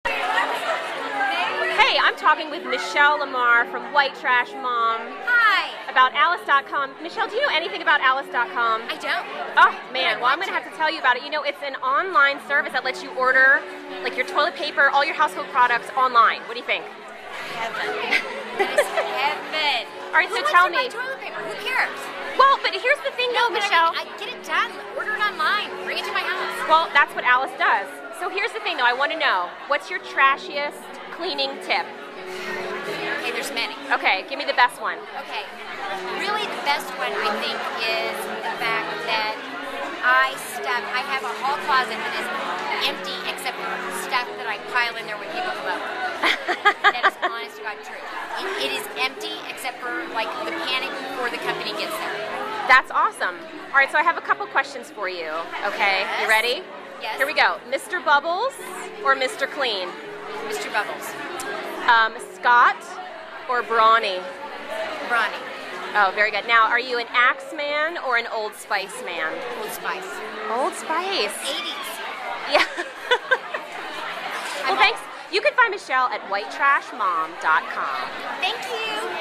"Trashiest" Cleaning Tip
Michelle Lamar (@michellelamar) of WhiteTrashMom.com shares her "trashiest" cleaning tip to Kristen Chase at BlogHer '09.